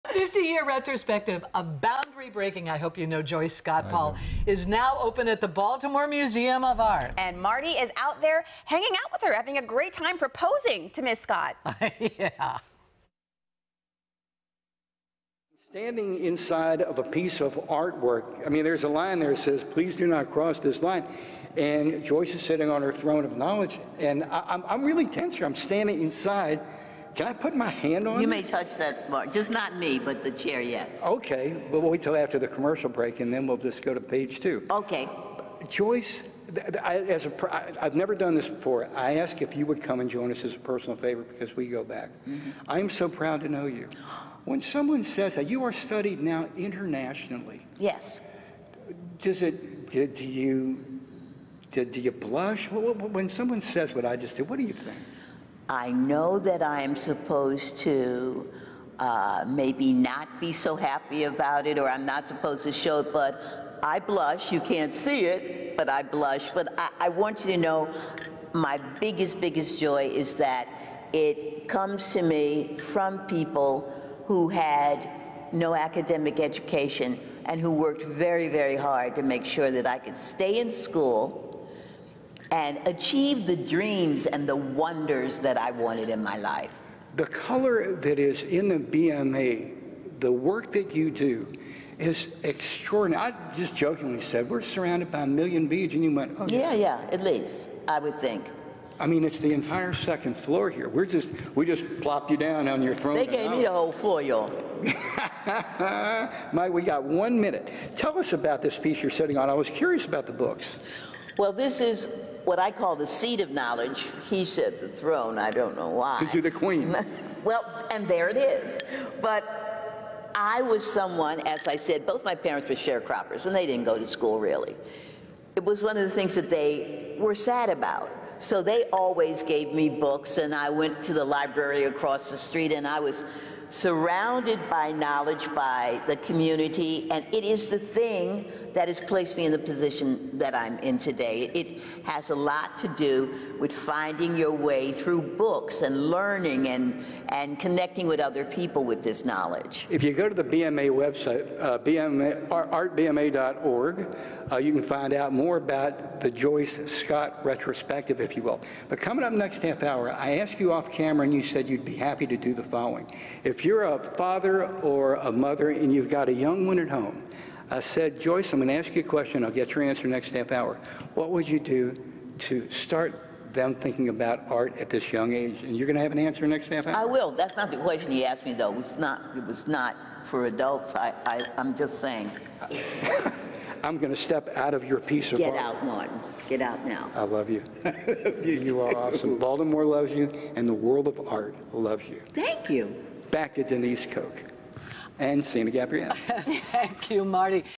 Renowned Baltimore-bred artist Joyce Scott discusses her work at the Baltimore Museum of Art retrosp
The astonishing virtuosity and ingenuity of Joyce Scott’s work in every medium seamlessly coalesces with her lifelong vision to confront racism, sexism, classism, and “all the ‘isms’ society offers” through impish and audacious humor, expressions of beauty, and a humanistic engagement with global events, the Baltimore Museum of Art says.